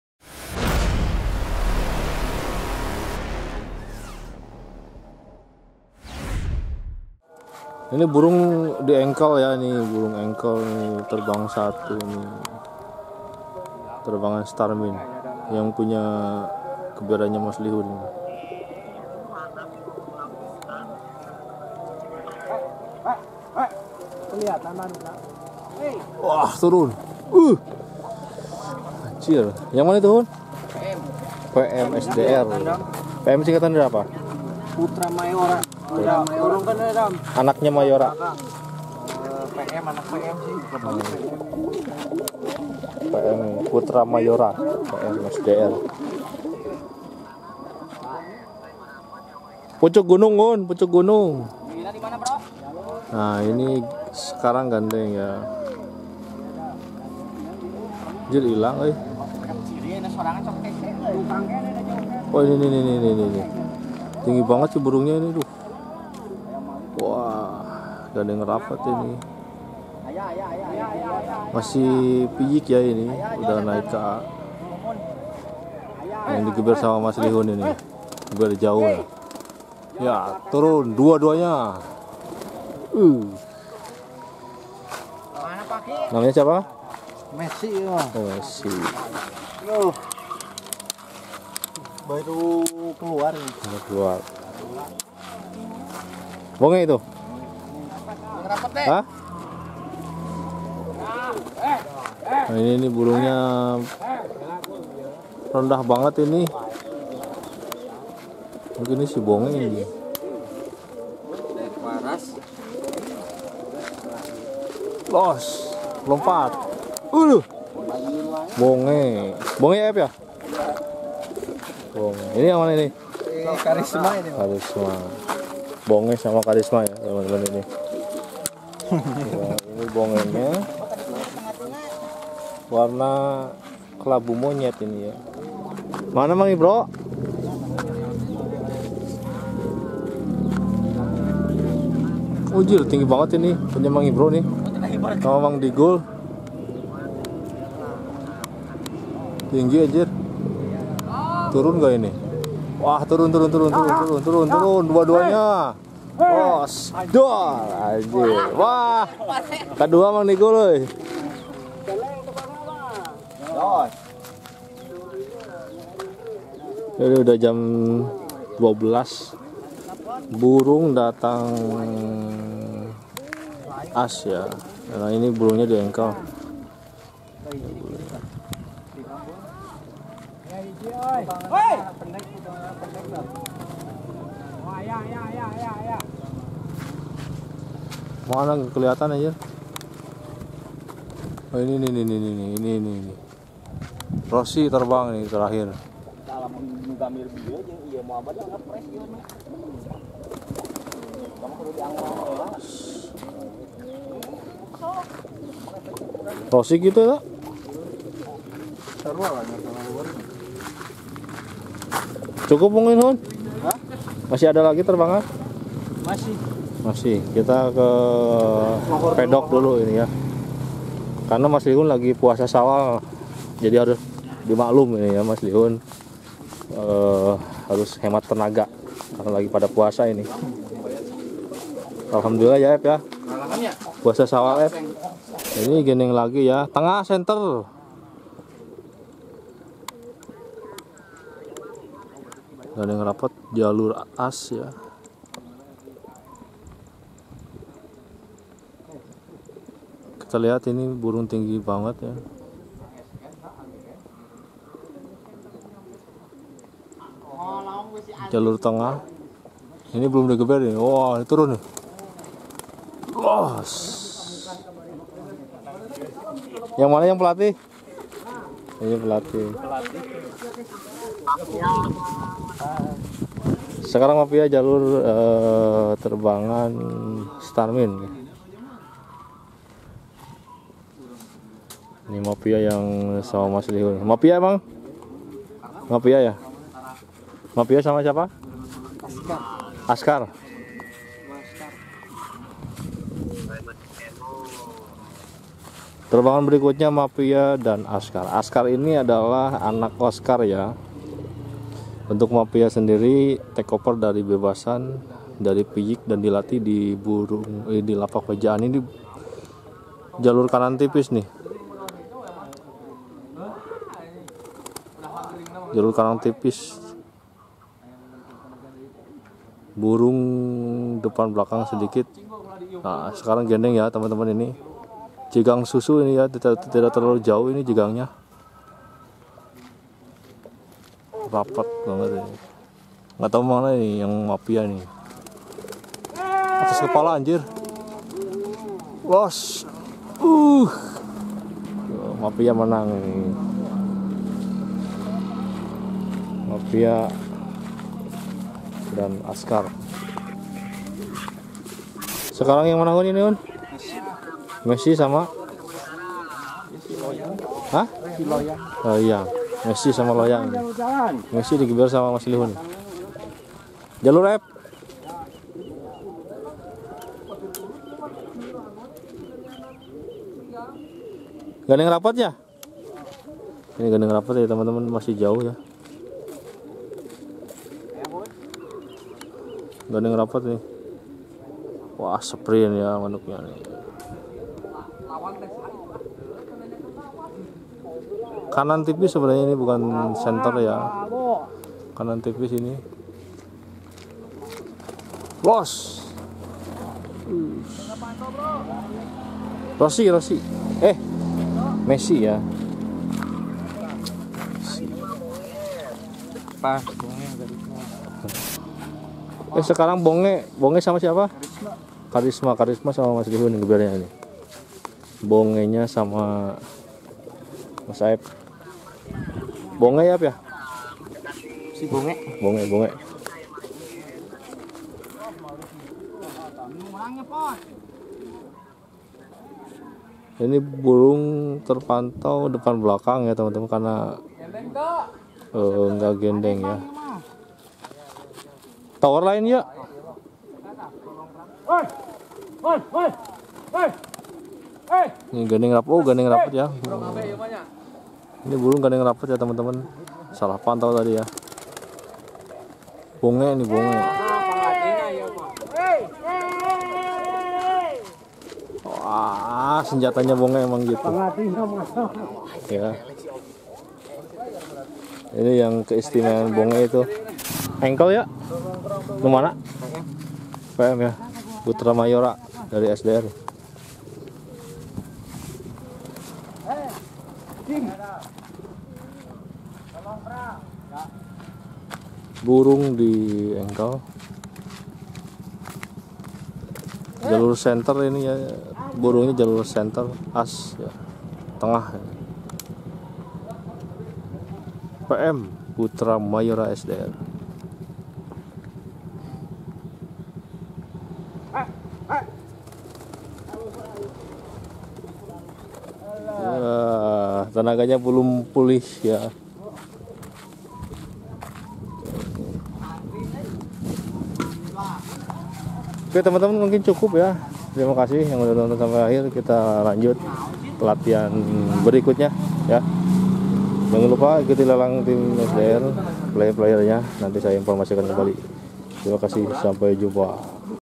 BONGE SDR MULAI ENAK KERJANYA - LATIHAN PLAYER MUDA TIM SDR CIAMIS PERSIAPAN LOMNAS PMTI 2024
Buat kalian yang ingin meminang piyikan atau bahan untuk player dari Tim SDR, bisa langsung hubungi aja nomer berikut:
Nomer Resmi Tim SDR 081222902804

ONEX MERPATI @produksi2024